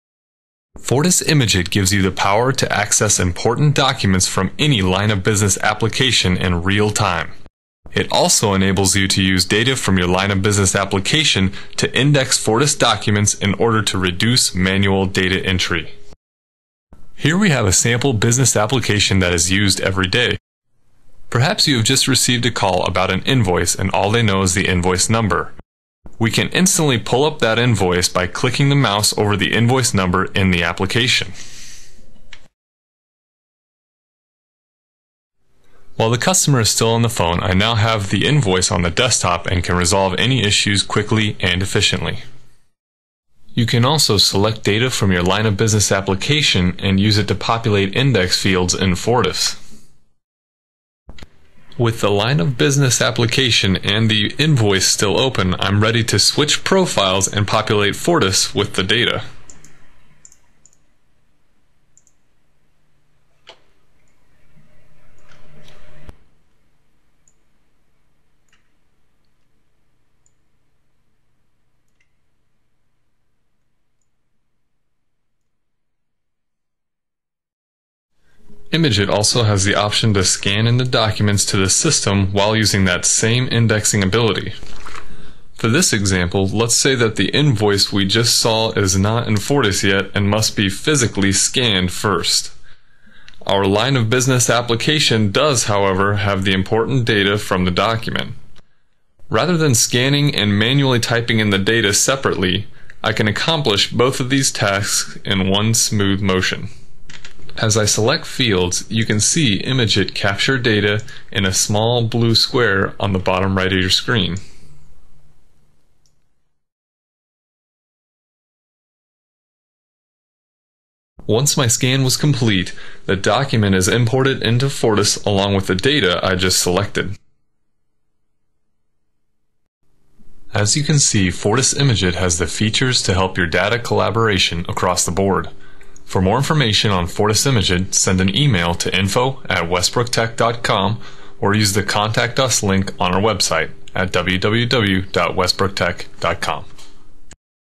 Fortis ImageIt
Seamlessly link documents in Fortis with other business software